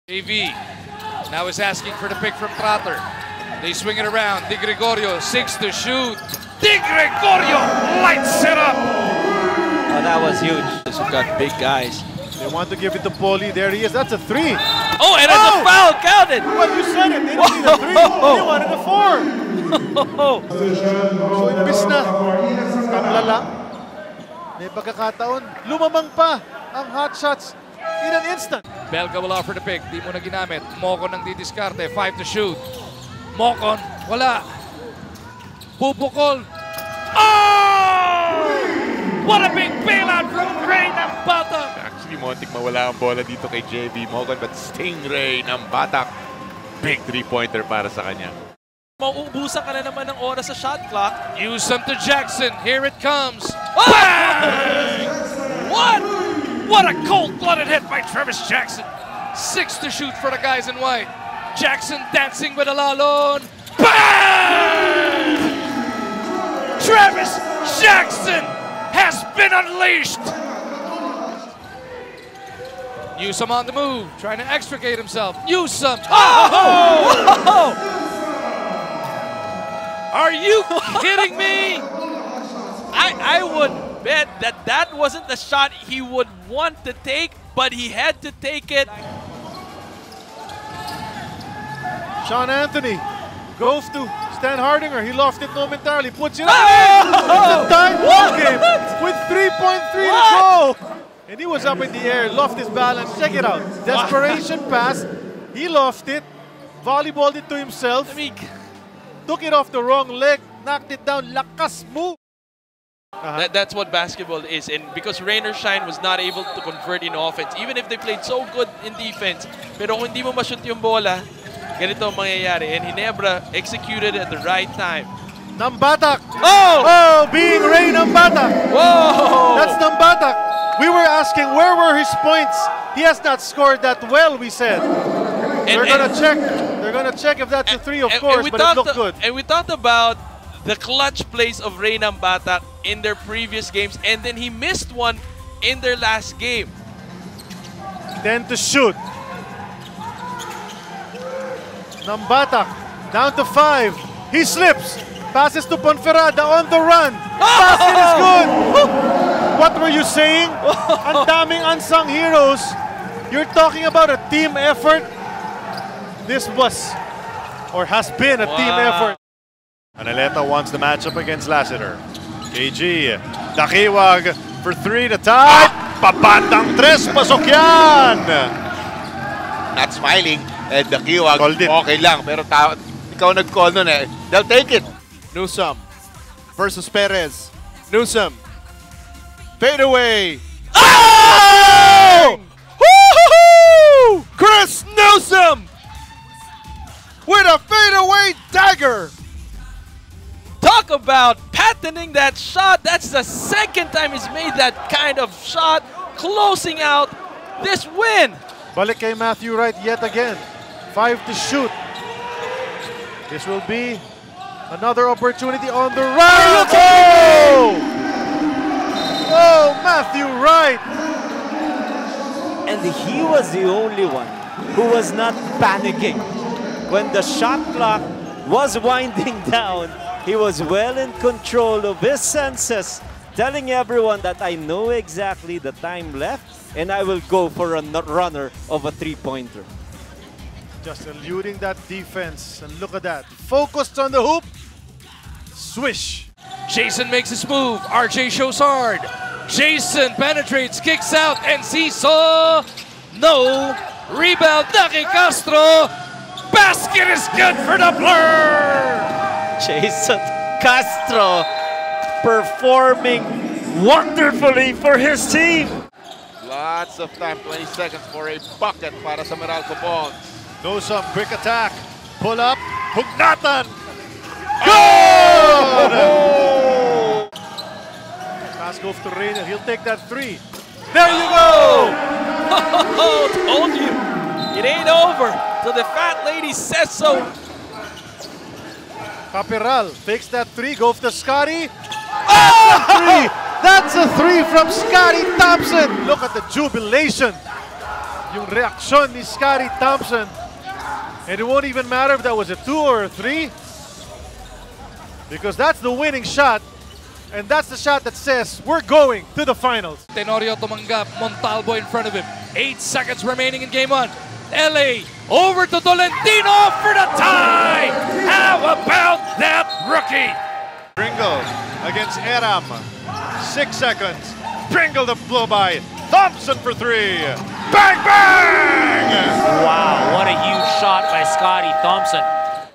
PBA Best of Clutch Plays | PBA Bubble 2020
"BANG!" 😤

Check out the most Clutch Plays of the 2020 PBA Philippine Cup!